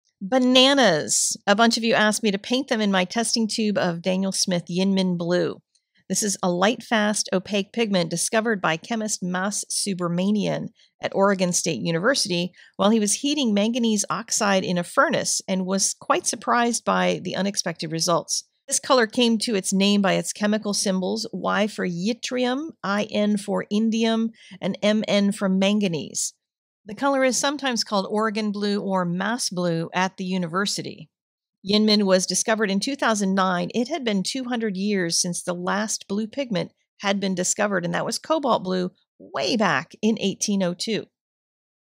YInMn Blue Banana!
This color isn't currently in production at Daniel Smith - they're deciding whether it's worthwhile, as it's SO expensive, would you buy a pricey little tube to get this color?

A R T V E N T U R E N E T W O R K
A R T S U P P L I E S